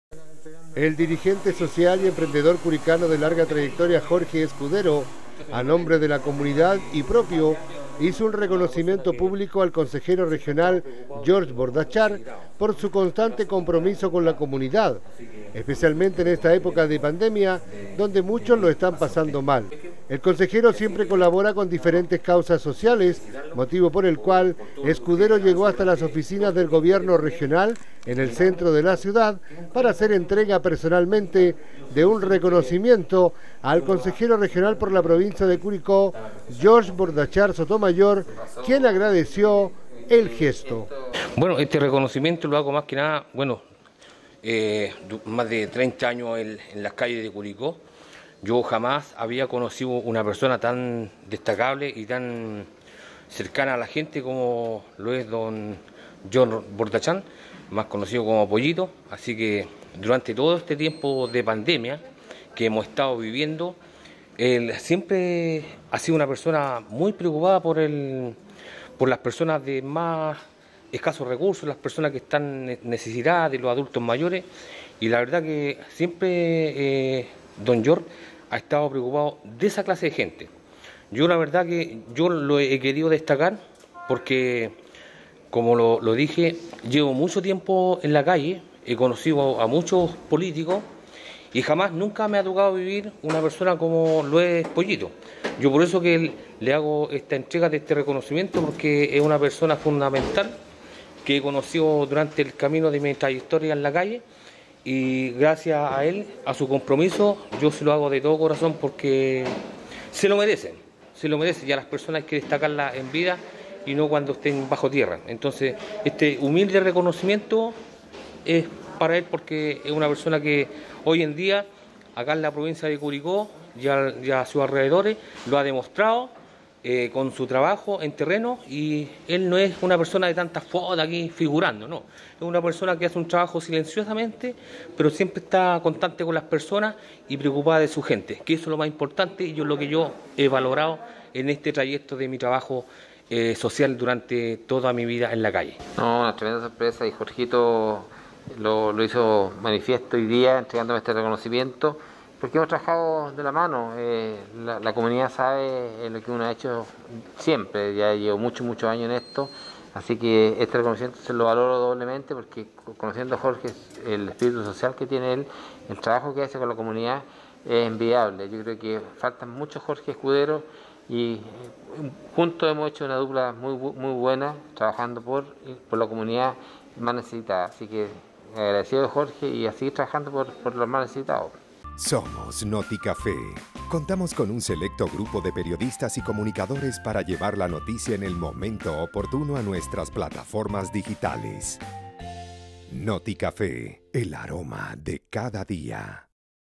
Destacan trabajo social en pandemia del Consejero Regional George Bordachar
El dirigente vecinal Jorge Escudero a nombre de la comunidad y propio entregó un público reconocimiento al trabajo social  desarrollado en pandemia por el Consejero Regional George Bordachar Sotomayor ayudando a la comunidad de toda la provincia de Curicó